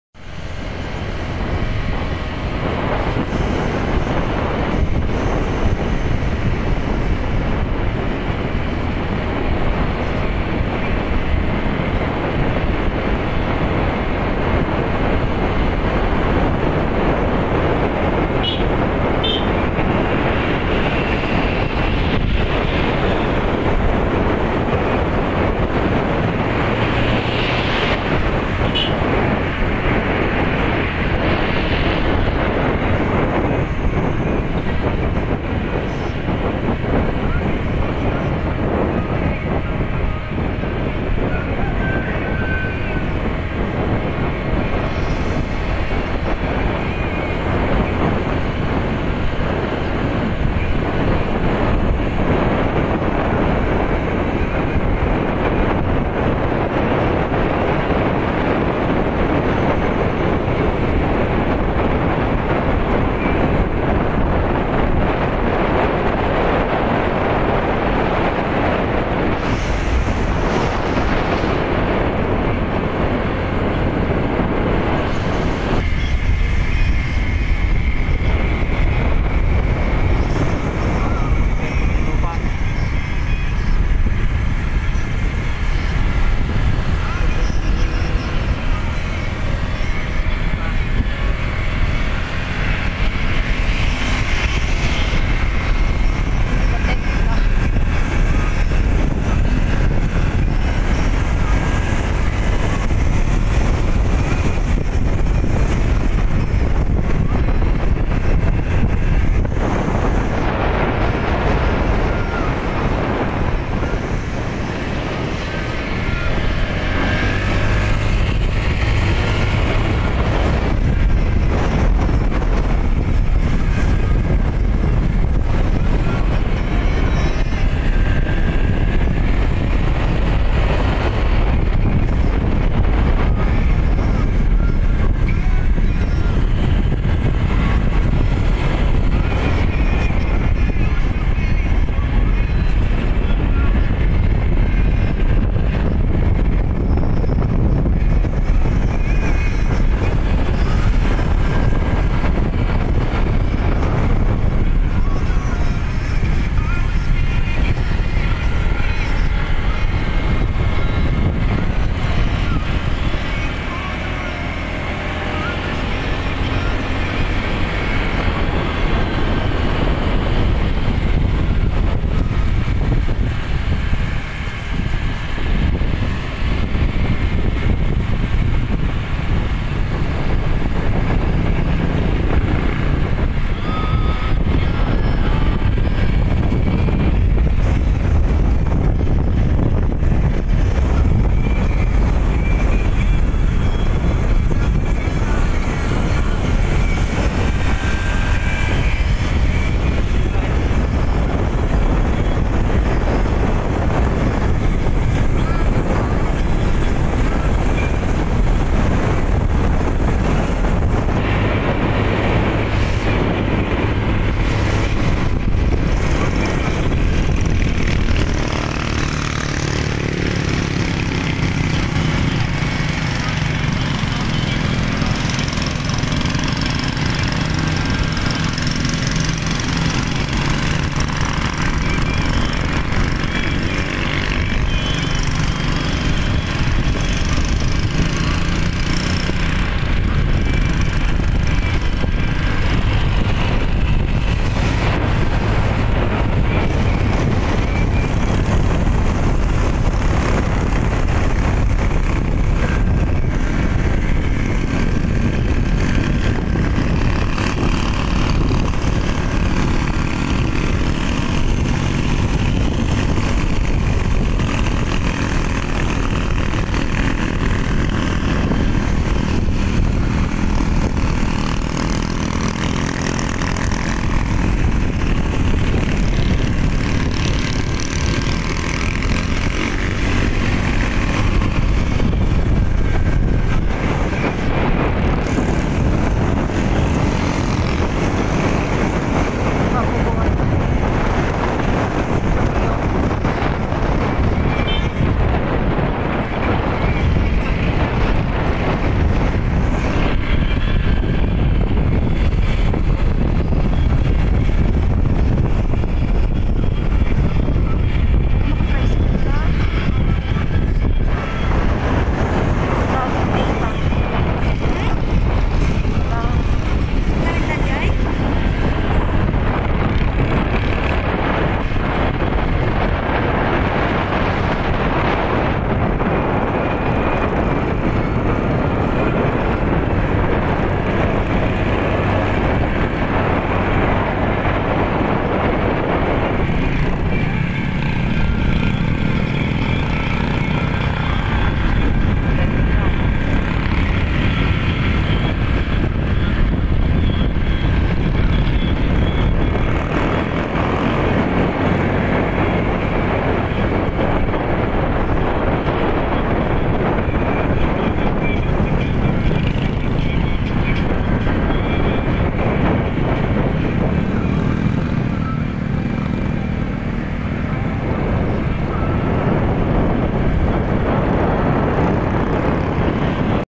Mountainous Rides-Quirino To Aurora Via Nagtipunan,Quirino-Dinadiawan,Aurora Road Part 1/3
Long Ride from Maddela,Quirino to Dinadiawan,Aurora
Watch the Beautiful Mountain Views and the Amazing Forest of many trees